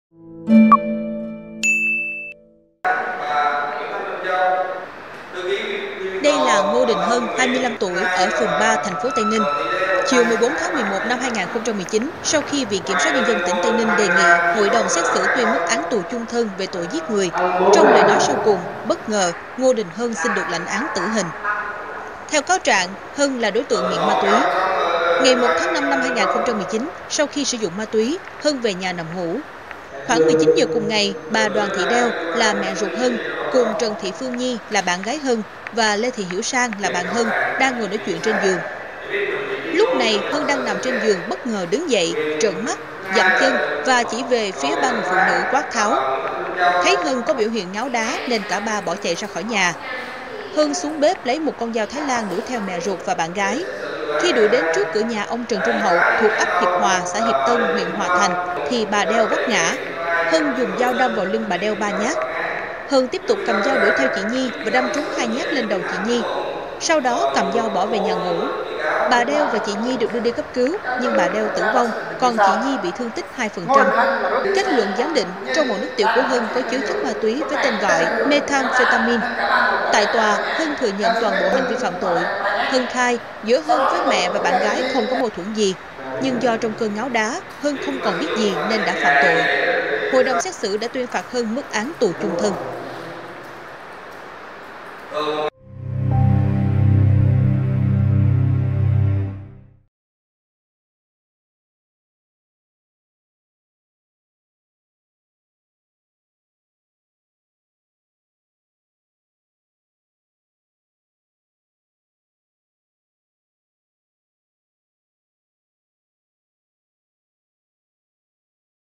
Thanh niên lên cơn “ngáo đá” giết mẹ và bạn gái, xin được lãnh án tử
Chiều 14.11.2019, sau khi Viện KSND tỉnh Tây Ninh đề nghị hội đồng xét xử tuyên mức án tù chung thân về tội giết người, trong lời nói sau cùng, Ngô Đình Hân (25 tuổi, ở P.3, TP.Tây Ninh) bất ngờ xin được lãnh án tử hình.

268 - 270 Nguyễn Đình Chiểu, Phường 6, Q.3, TP HCM